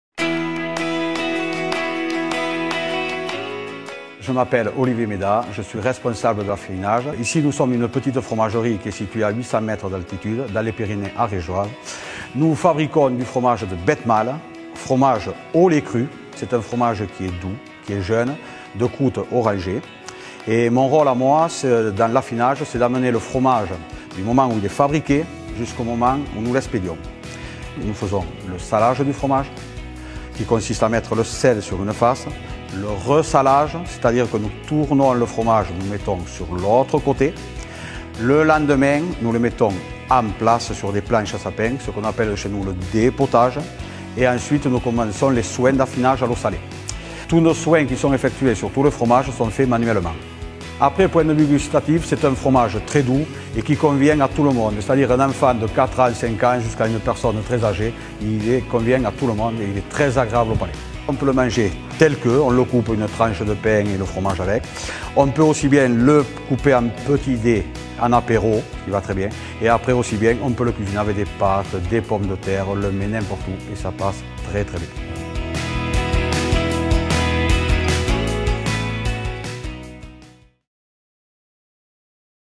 BETHMALE - VU PAR UN MAITRE AFFINEUR
Olivier MEDAT, responsable de l'affinage, nous présente les caractéristiques du Bethmale fabriqué dans les Pyrénées ariégeoises. Du moment où le fromage est fabriqué jusqu'à son expédition, les maitres affineurs s'occupent de ce fromage dit "à pâte orange"